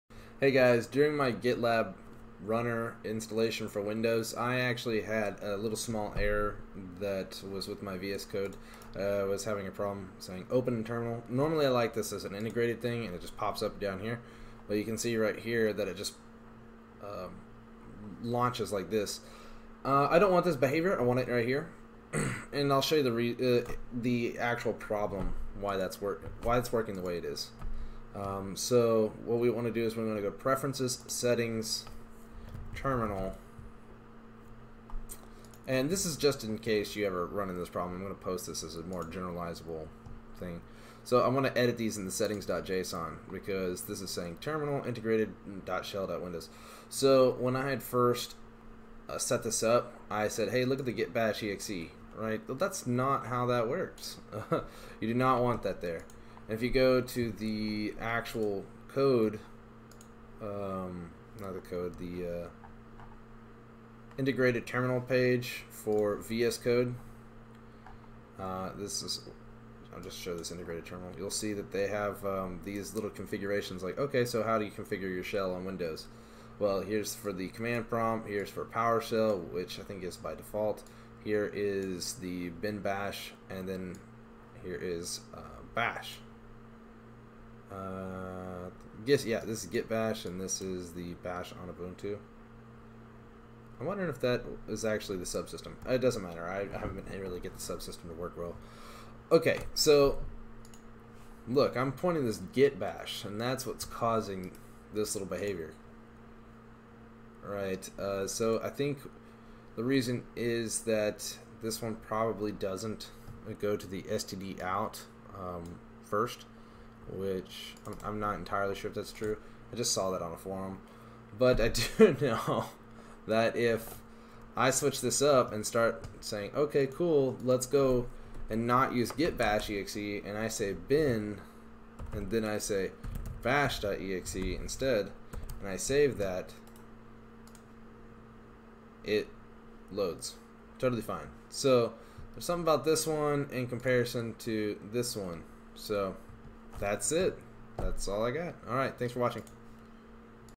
VSCode Integrate Terminal Launching Externally Fix
This video is a companion to the gitlab CI/CD Series part 1 video.  During that video my git-bash terminal kept popping out of VSCODE which I didn't want.  I wanted it integrated and it was already set as integrated.   Well it turns out that not every terminal can be set as integrated and the location of the terminal must be a specific one.  So in this video I show the wrong and right path to the git-bash terminal to be used.